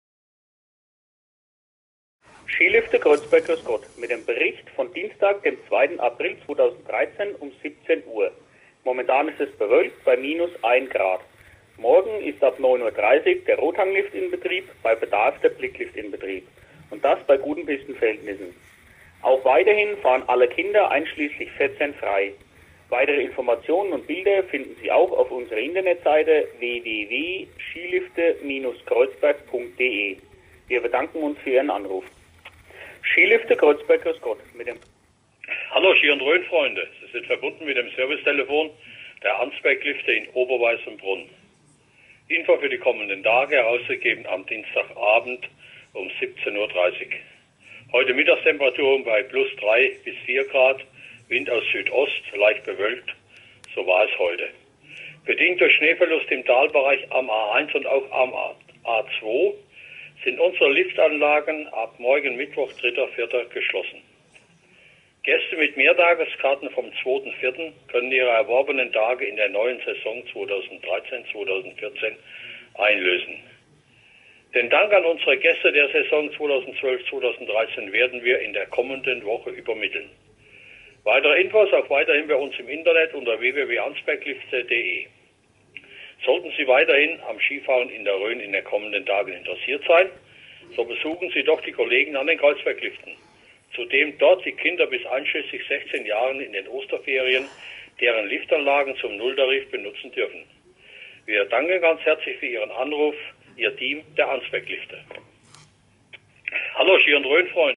Schneetelefon Rhön vom 2.4.2013
Telefon-Ansagen der Kreuzberg- und Arnsberglifte vom 2. April 13

Schneetelefon Vorwahl 09772
Kreuzberg  433
Arnsberg   277